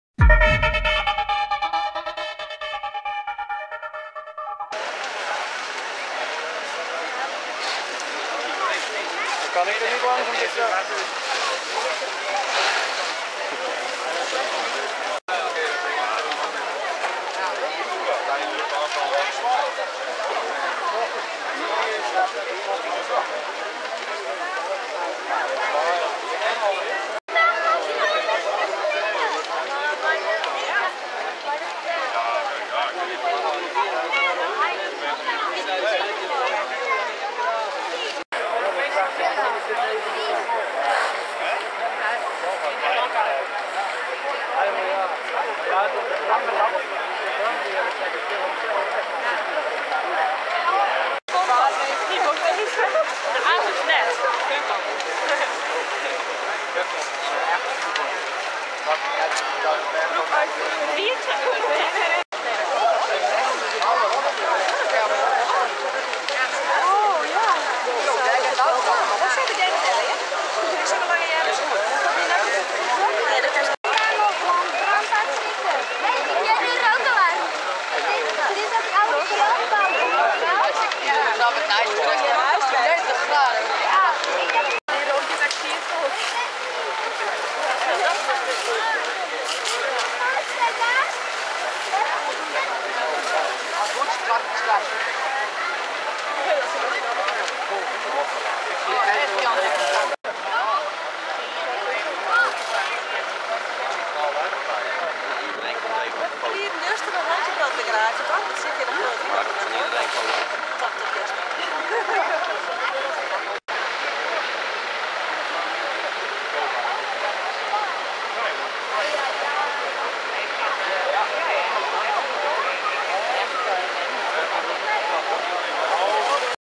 15-09-2012 Drachten - Grote brand verwoest slooppand
DRACHTEN -- Zaterdagmiddag ontstond rond kwart over twee brand in het voormalige Shell gebouw aan de Splitting.

Het gebouw op de kruising met de Zuiderdwarsvaart stond al snel volledig in brand en stortte in. Het korps van Drachten kreeg bij de bluswerkzaamheden assistentie van de brandweer van Beetsterzwaag terwijl het korps van Burgum een grootwatertransport leverde.

De politie zette kruisingen rondom het brandende pand af waardoor een grote verkeerschaos ontstond. De oorzaak van de brand is vermoedelijk brandstichting.